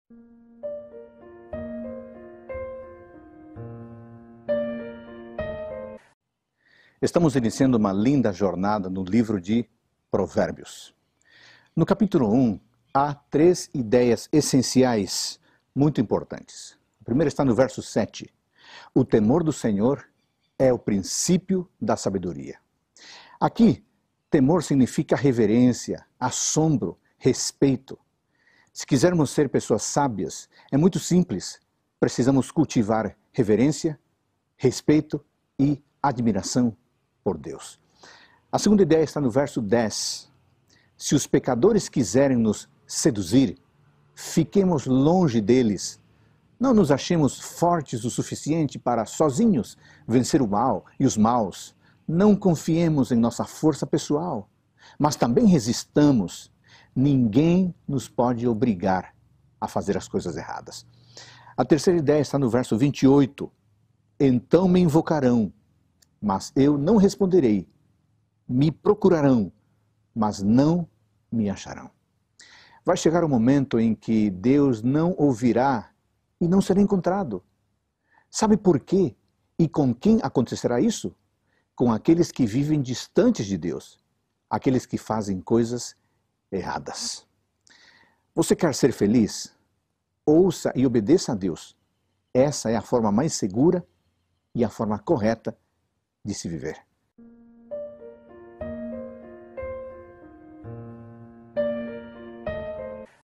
Provérbios 1 - Reavivados por Sua Palavra
Se quisermos ser pessoas sábias, é muito simples: precisamos cultivar reverência, respeito e admiração por Deus.